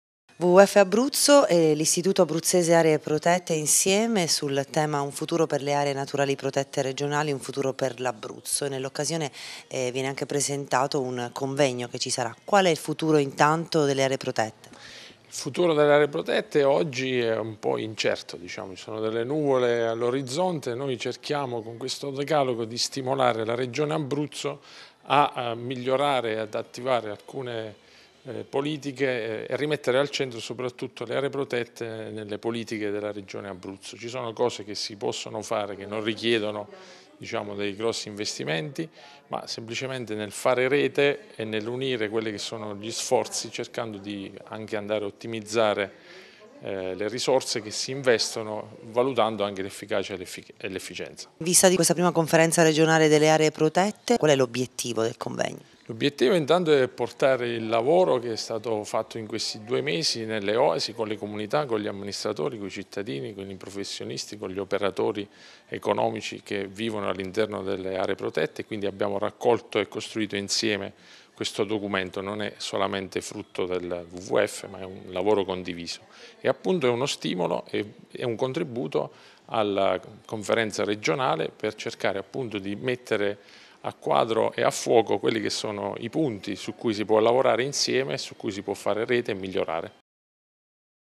Le OASI WWF verso la conferenza regionale delle aree protette
Il wwf Abruzzo e l’Istituto Abruzzese per le aree protette insieme per dare futuro alle aree naturali protette, per fare economia e rilanciare la regione.